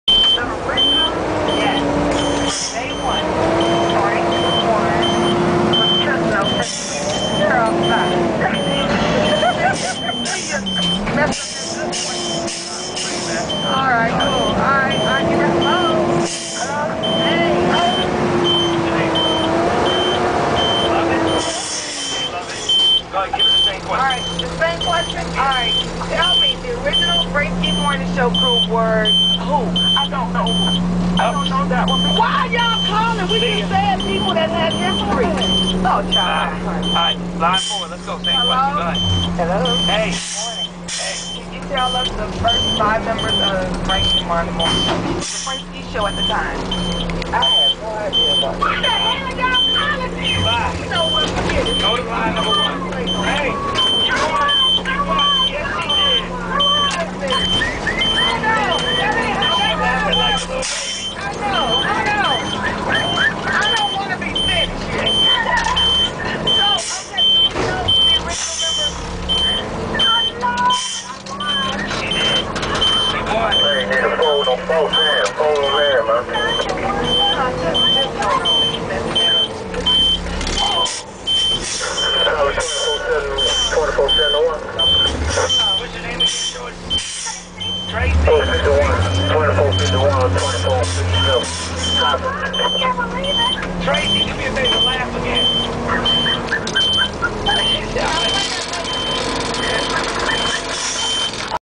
Rainy Day Monday 10-22-07
Servicing two roll-out compactors at an apt complex. I have to block off both lanes of traffic to service these, thus the need for the annoying flashers.